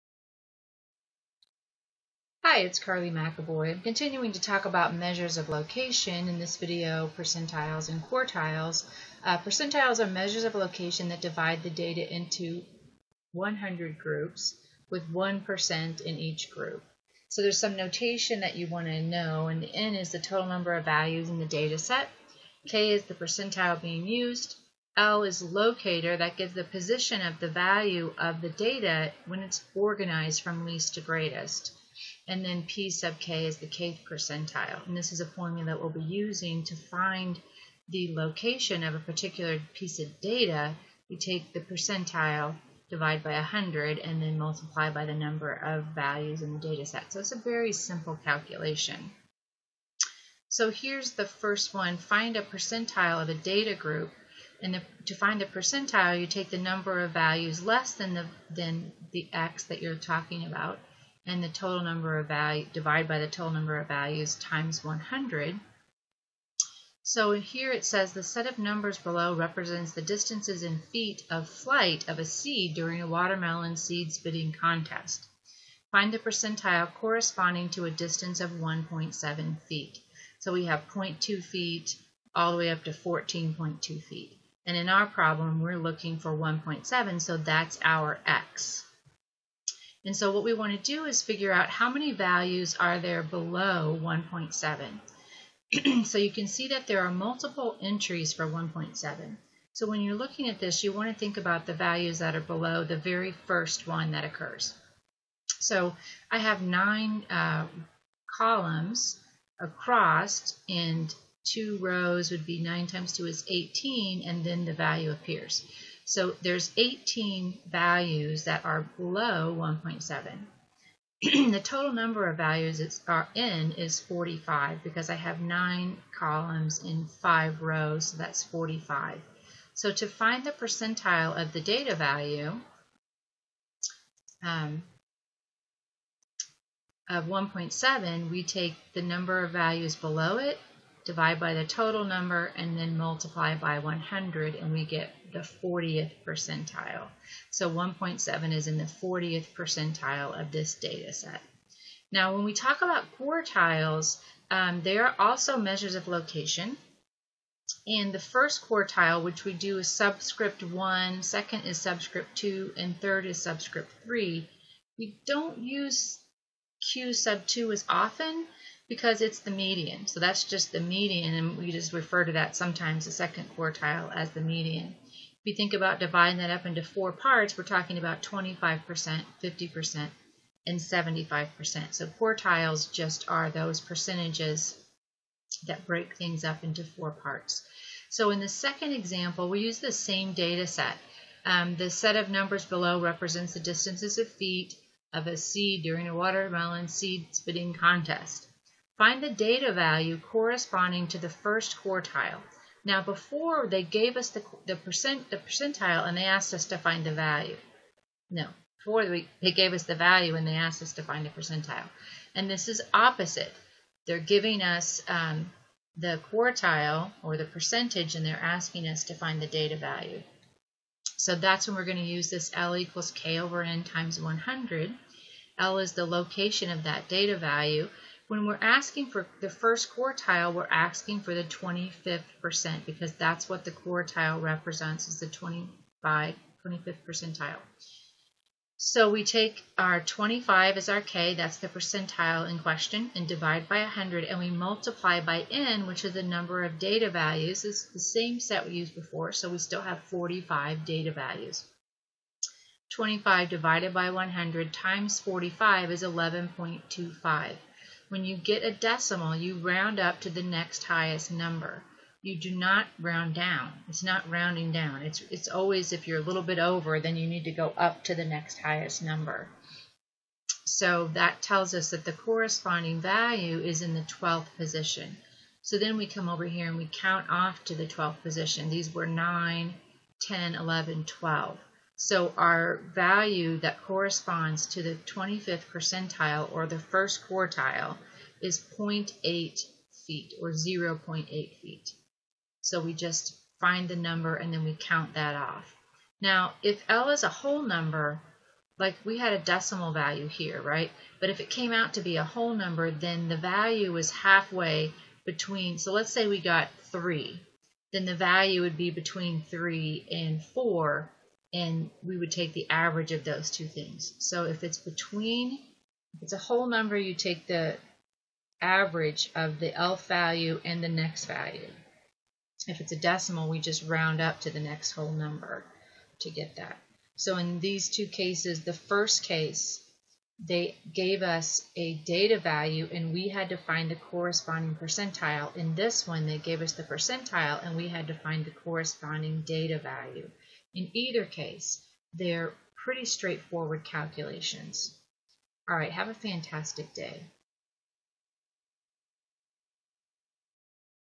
Statistics: Percentiles
Find the data value given the percentile and find the percentile given the data value.